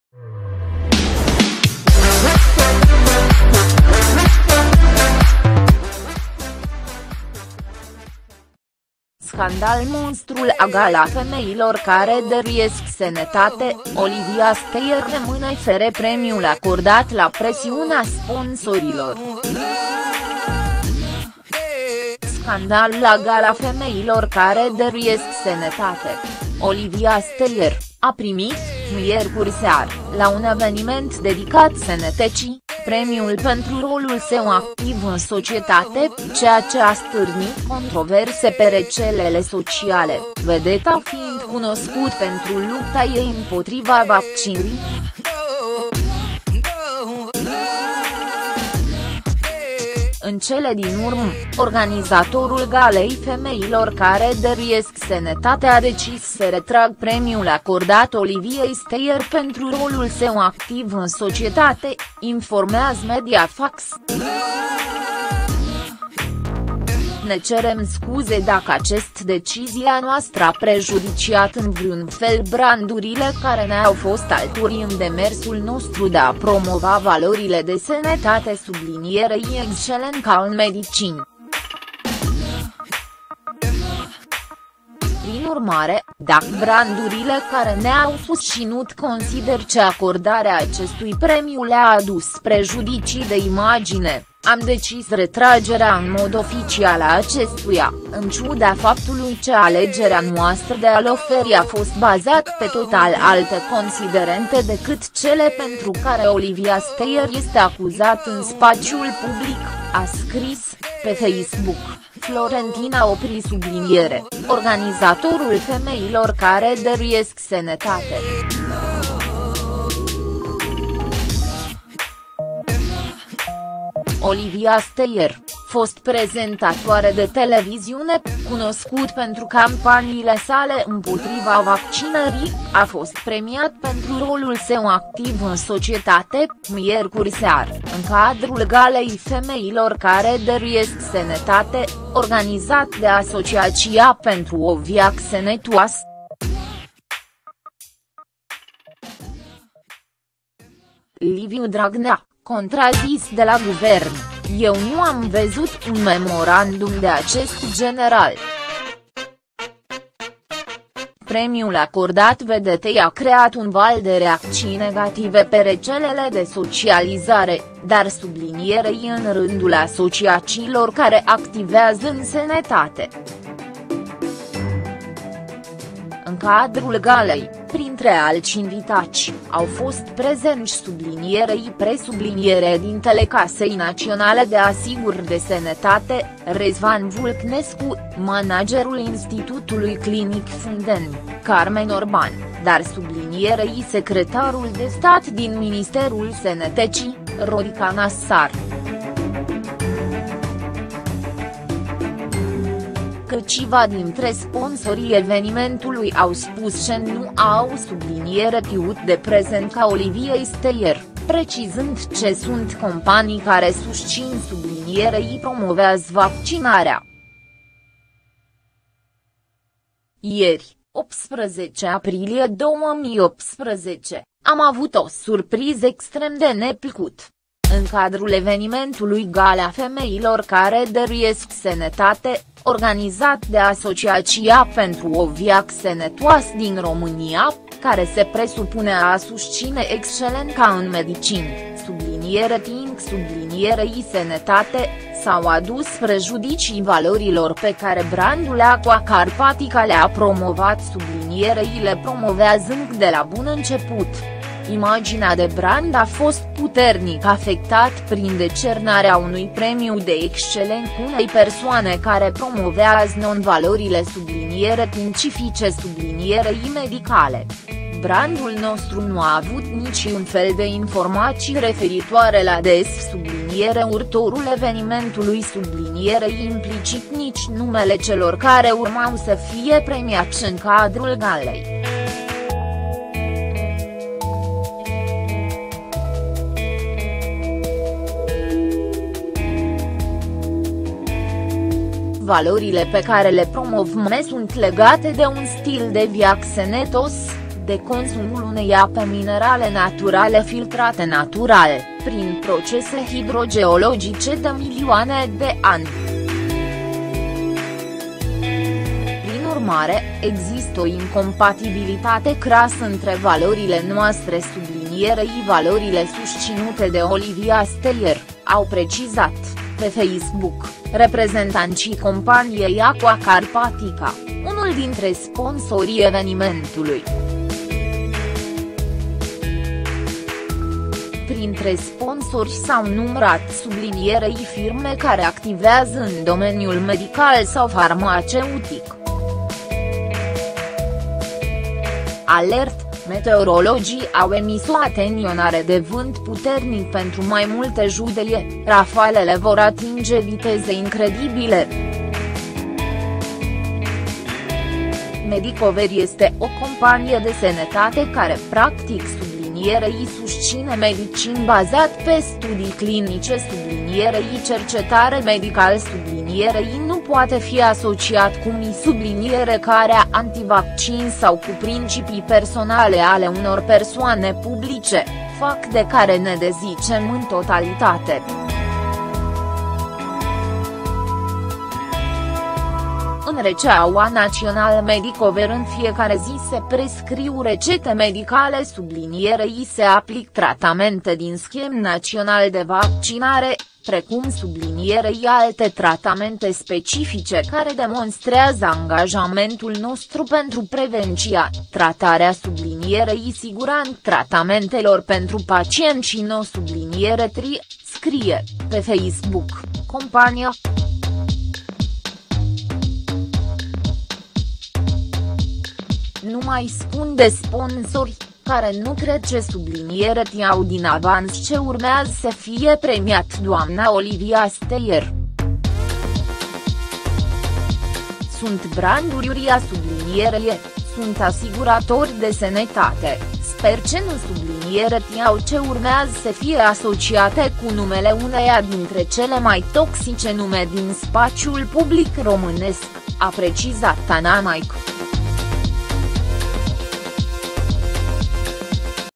Scandal MONSTRU la Gala Femeilor care Dăruiesc Sănătate: Olivia Steer rămân
Scandal MONSTRU la Gala Femeilor care Dăruiesc Sănătate: Olivia Steer rămâne fără premiul acordat la presiunea sponsorilor